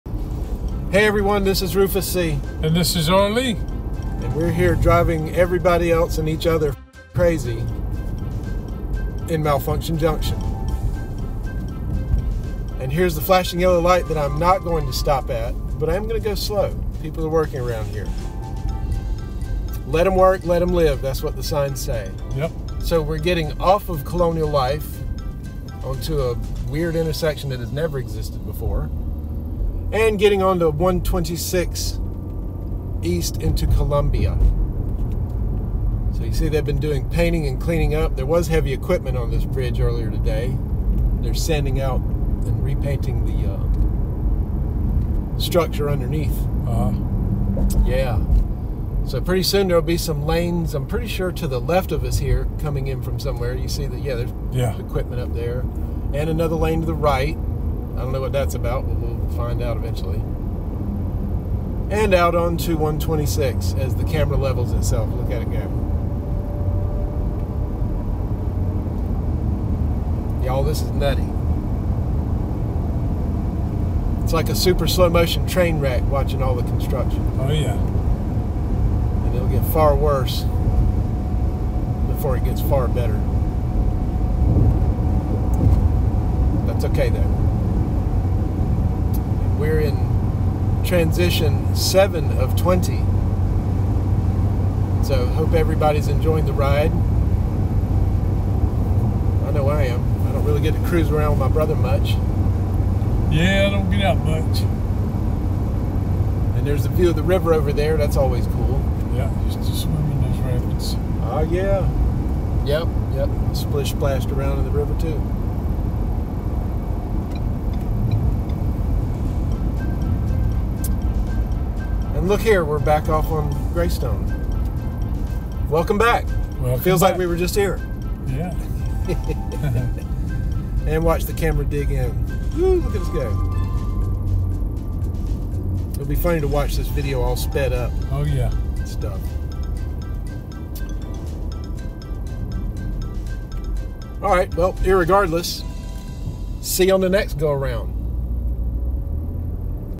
07-20 Malfunction Junction Recall Colonial Life Blvd to I126 East Columbia, SC
This is a video series called "21 of 20." We are recalling the "Malfunction Junction" at Interstate 26 and Interstate 20 in Columbia, SC before all the new construction called, the "Carolina Crossroads" totally changes this system, as well as the surrounding entrances and exits.